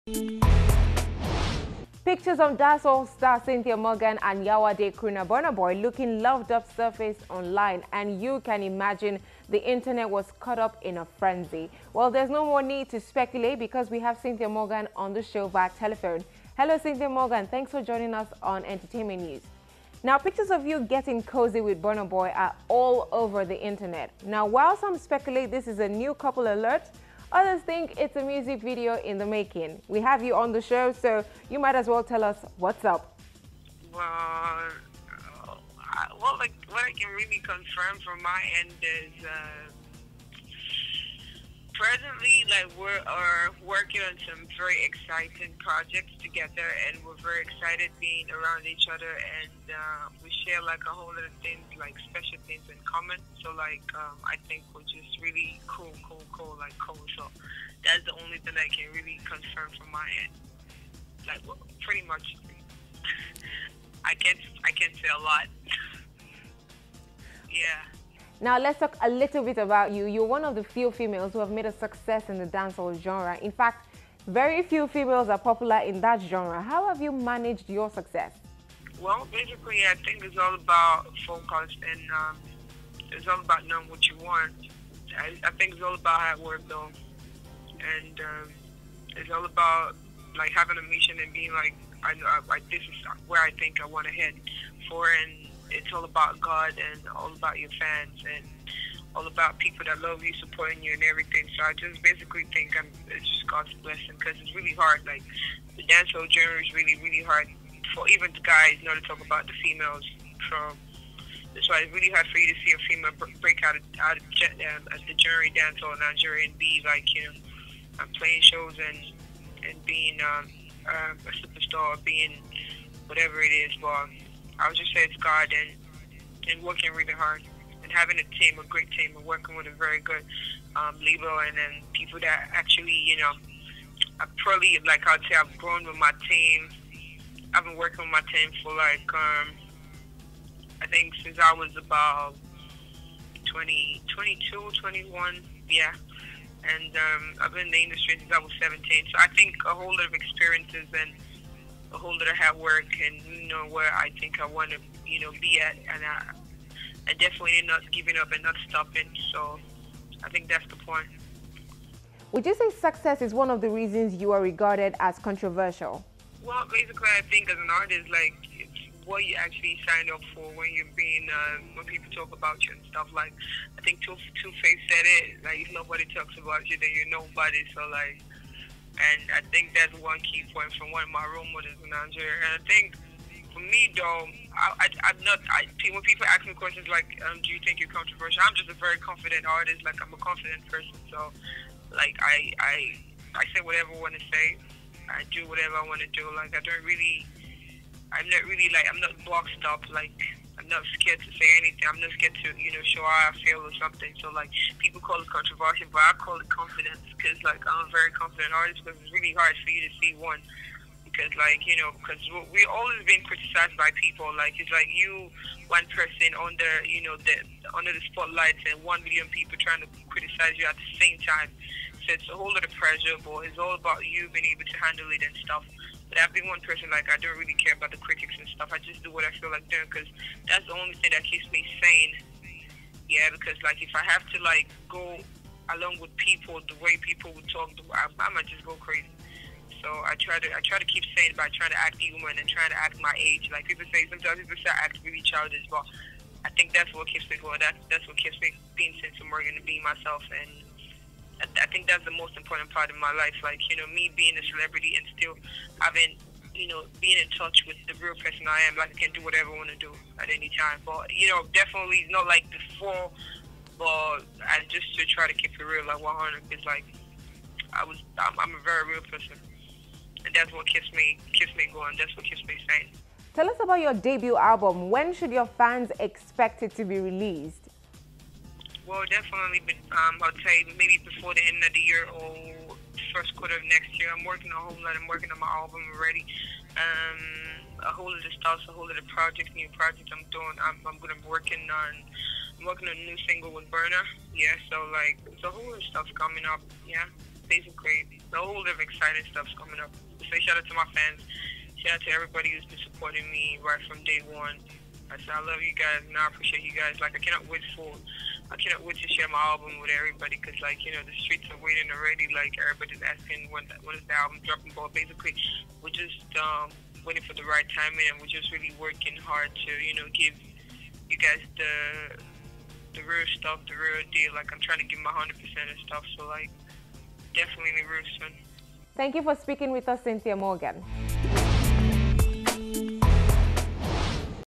EN: I Don't Care What Critics Say - Cynthia Morgan
Dancehall star, Cynthia Morgan is one of the few successful female artistes in the music genre.